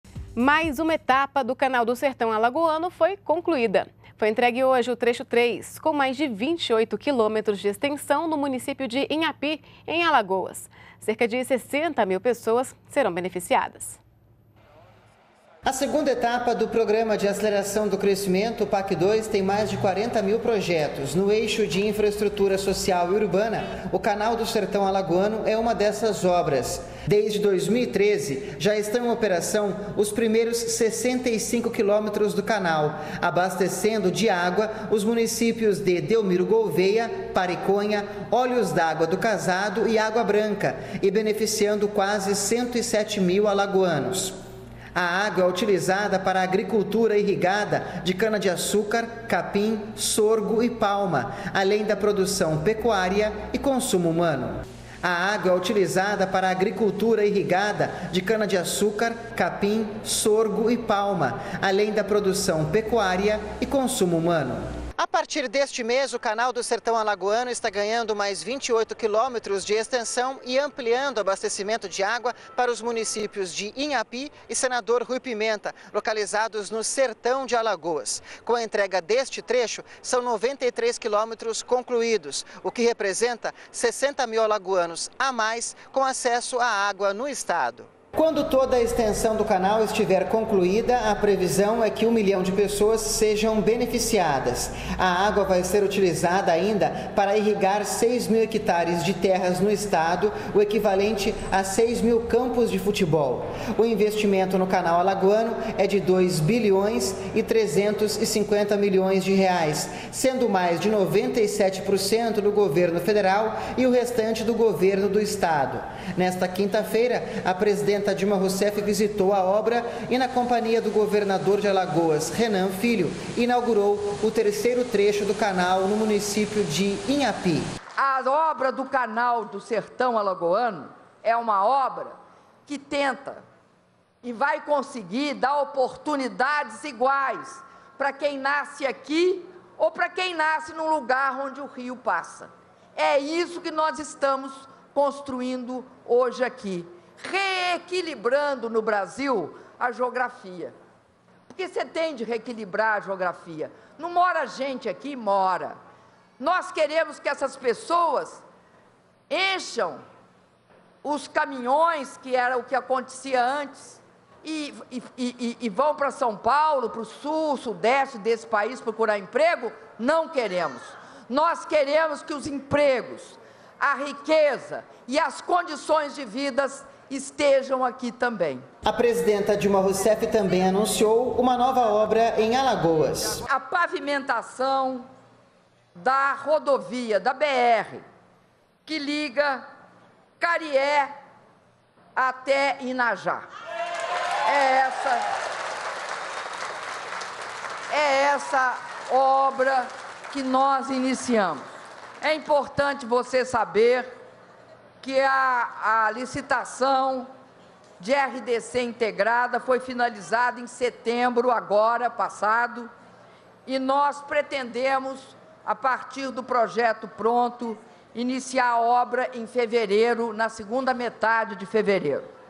Dilma Rousseff entrega novo trecho do canal do Setor Alagoano
NBR NOTÍCIAS - 05.11.15: Foi entregue nesta quinta-feira (5) mais uma etapa do do canal do Setor Alagoano. O trecho 3 tem mais de 28 km de extensão no município de Inhapi, localizado em Alagoas. Cerca de 60 mil pessoas serão beneficiadas pela obra, que conta com recursos do Programa de Aceleração do Crescimento. A segunda etapa do programa tem mais de 40 mil projetos no eixo de infraestrutura social e urbana, como o canal do Setor Alagoano.